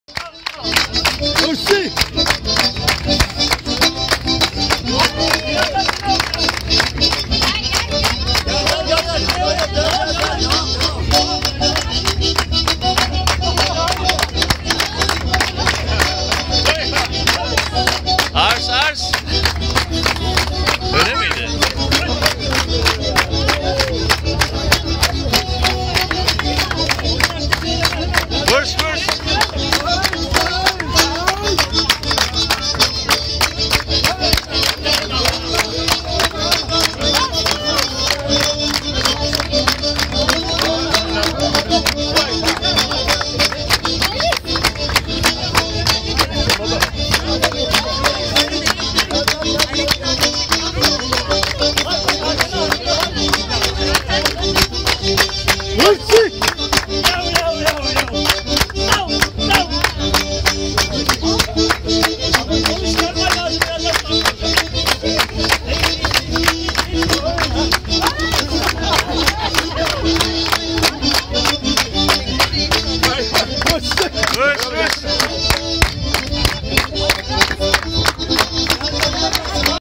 Istanbul Boğazında Çerkez düğünü - Filiz & Hayrettin
Filiz & Hayrettin evlendi. 31.Temmuz 2016